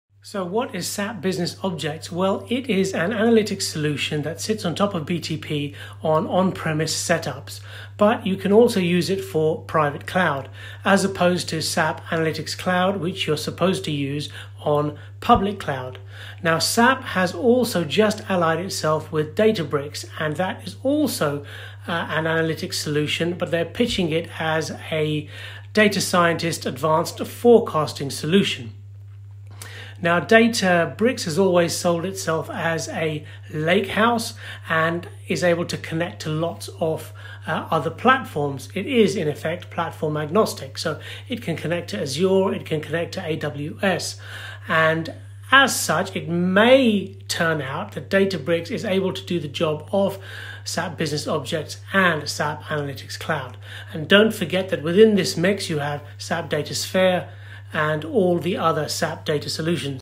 What is SAP Business Objects (Business Intelligence) in SAP S/4 HANA? SAP Analytics Cloud/Databricks
; @MarkBashirYoutube ; Based in London, UK.